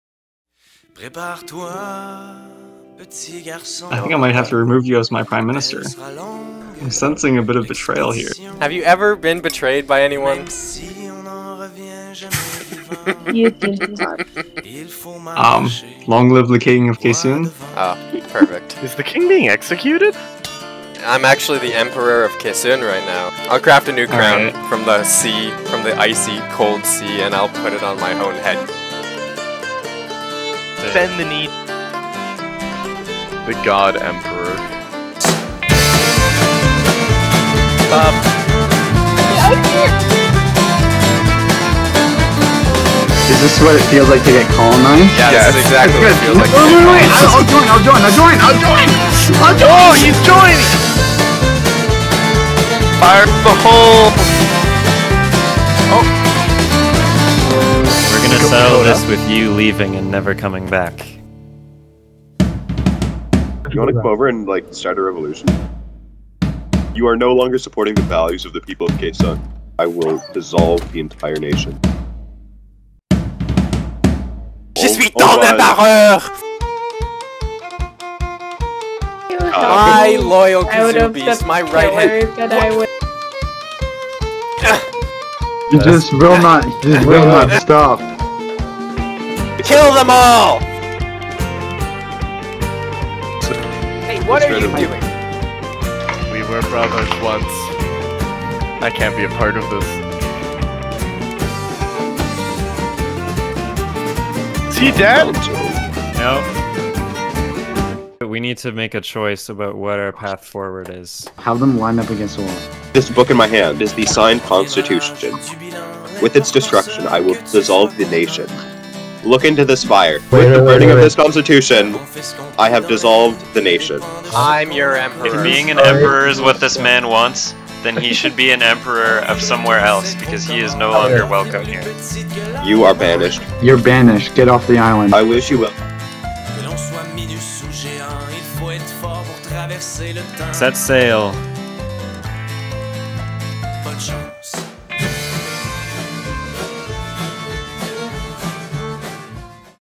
The Rise, Conquest, and Fall of Empereur Brob de Quéçeune
Brob was not content with the stagnation of his great city, and blamed it on Roi Al. By the lighthouse near the North gate of Ville de Quéçeune, Brob constructed a guillotine. He met with the roi. Sensing betrayal, Al attempted to flee. Brob blocked the roads and forced Al into the machine. With the final words "Long live the king", Al was executed and Brob declared himself the new roi. Deviously, Al had hidden away the crown before he died, so Brob forged a new one. The kindom was to be reformed into an empire under the new crown, and Brob had his sights set on expansion.

First, Brob headed East to Killikaura. There was Starling, amidst the ice in the once-thriving city of Raavik. Destitute, Starling bent the knee peacefully and declared Killikaura a territory of Quéçeune. After flying a colonial flag over the main gate they re-armed Starling, then headed to Valaura. They planted new flags and attempted to incorporate this land into the empire, but were met with resistance from Enclave troops who claimed to want a peaceful island where they could stay secluded without worry of war. Desiring a swift, bloodless takeover, Brob negotiated with the Enclave and with Raellka. The Enclave demanded that the imperial settlers vacate Valaura, but Raellka quietly decided that if Starling was to be executed for breaking her laws, she would agree to become a satellite territory. Back in Ville de Quéçeune, a crowd met for Starling's confession and execution. As the guillotine fell, Raellka pledged her allegience. The mercenary Mastercrooft was among the audience, and Brob recruited him to aid in Quéçeune's next conquest: Maple Creek. A war band of Brob, Starling, Raellka, Mastercrooft, and Sikuqtaq ally Ali ventured North, passing through Mooseton on the way to Maple Island. They crossed onto the Eastern side then pushed West into Tuath. Dakota attempted to flee by ship, but Mastercrooft killed and pinned him down. In front of the gate, Brob forced Dakota to take down his own flag and replace it with one that paid tribute to the Empire of Quéçeune. The party returned to Ville de Quéçeune, took inventory, then also laid claim to Sushia. Empereur Brob had annexed all of Eastern Moose Island in one fell swoop, but he was hungry for more. They pushed west, into Enclave territory.

Arrows rained from the lake fortress as Quéçeune troops took shelter on the hill of the Eastern bank. They fired back and soon pushed across the lake into the Enclave. Once inside, it became clear that the Enclave had prepared for this scenario. The fortress was like a maze, and Quéçeune troops could not gain a foothold. Additionally, Mastercrooft betrayed Brob and refused to fight, resulting in Quéçeune troops being overwhelmed by guerilla tactics. Brob, maddened by a lust for power, was relentless. He kept throwing himself at the enclave even though success was no longer viable. His gear was confiscated, yet he and Starling persisted. He was thrown into the snowy hills to the North with nothing, yet was still convinced the invasion would end up successful. Kazoo, starting a revolution for peace back in Ville de Quéçeune, called for an end to the fighting. He did not stand for war and claimed it went against the ideals of the nation he knew. Brob, enraged, attempted to execute his once-trusted citizen, only to be beaten down and thrown out because he was now weak and corrupted and blind. The server rounded up Brob and the still-loyal Starling and decided that the conquest had to come to an end. Kazoo burned the crown and the constitution, and it was decided that Brob could remain emperor, but of a place where he could no longer cause harm. He and Starling were banished to a small island in the North West, and all conquered territory was returned. Quéçeune was dissolved and the city given to Moose Island. With oversight from Kazoo and Ali, Brob and Starling began fresh lives as castaways.